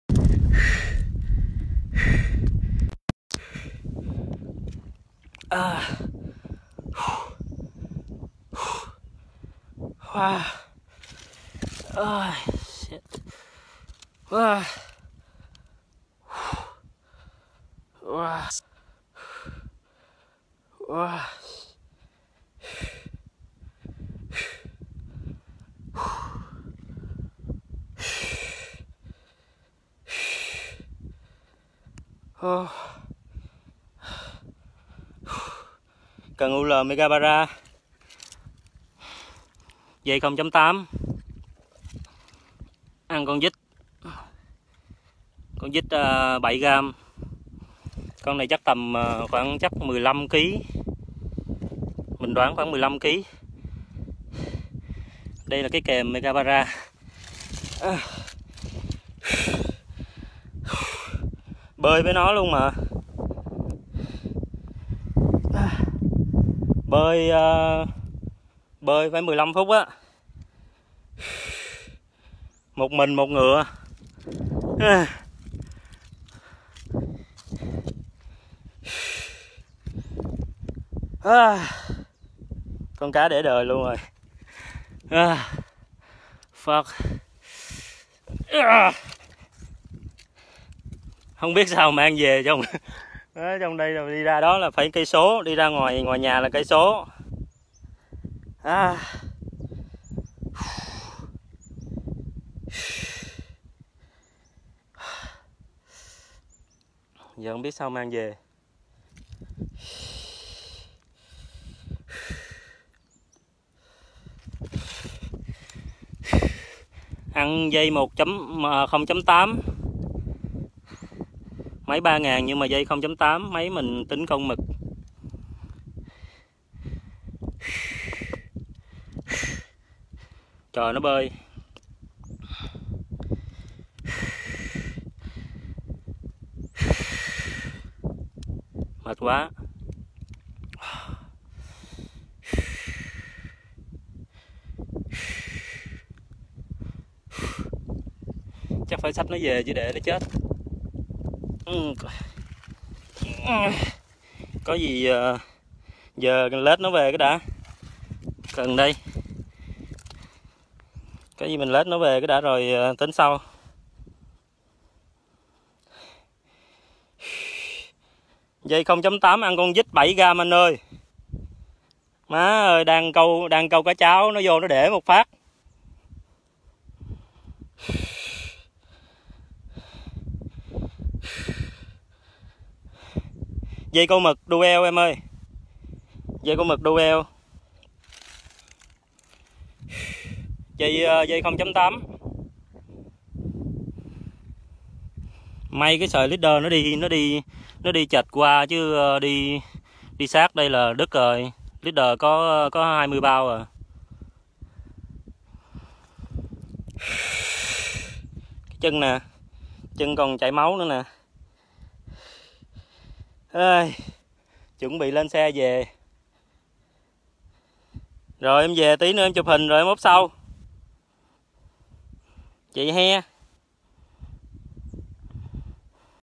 Live Stream: Monster Barramundi in Vietnam. Câu dính cá chẽm khổng lồ, 1 mình bơi theo để bắt lên bờ
Đang câu cá măng, bằng cần UL, dây 0.8 thì bất ngờ bị tấn công.
Rod: UL Megabarra, sẽ có sản phẩm chính thức vào tháng 4/2020
Line: Gosen 0.8
Lure: Micro Jig 7g
Hook: BKK #10
Reel: Daiwa Theory 2510
Phân phối sỉ và lẻ dụng cụ câu cá các thương hiệu Nhật Bản